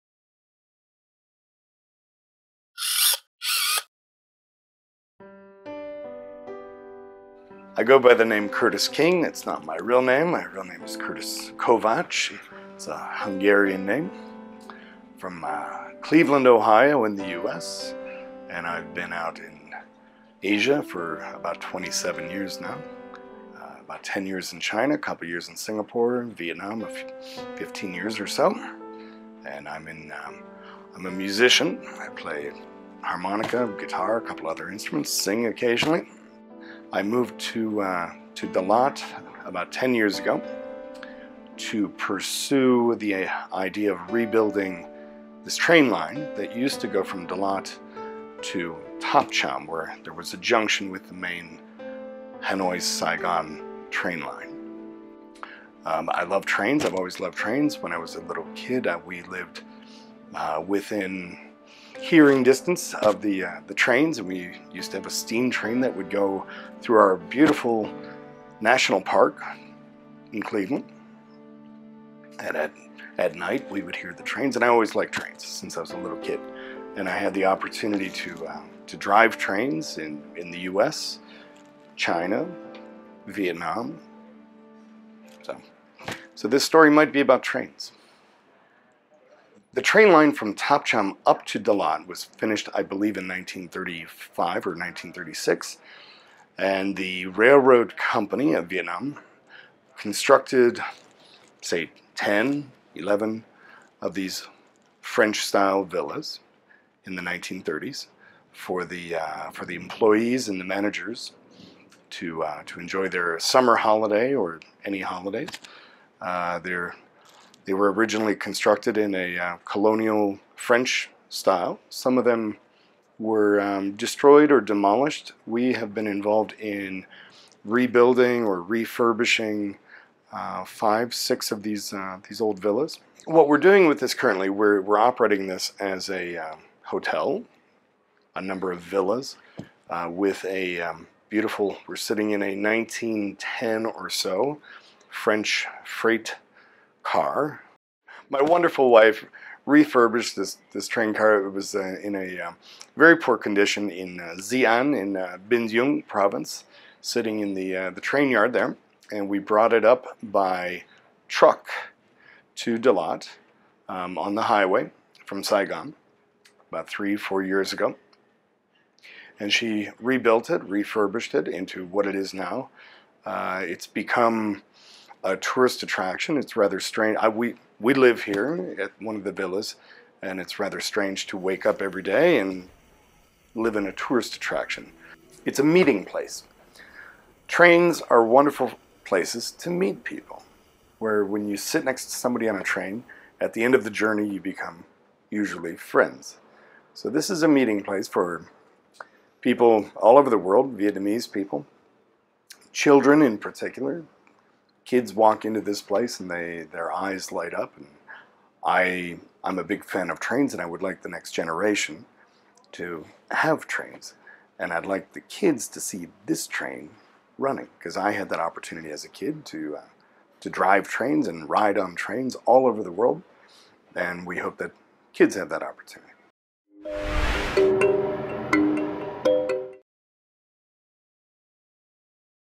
Curtis King - The Train Man of Dalat
AsiaLIFE sat down with a native of Cleveland, Ohio, USA that is attempting to rebuild a beautiful time capsule in the form of a train line in Dalat.